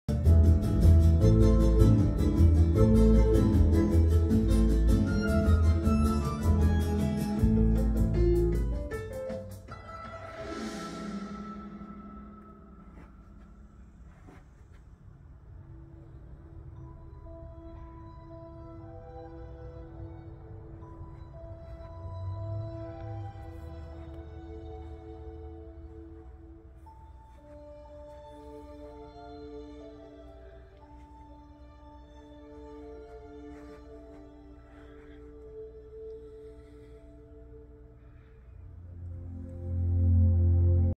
Hachette Andromeda
Short video of the completed Hachette Andromeda from Space Battleship Yamato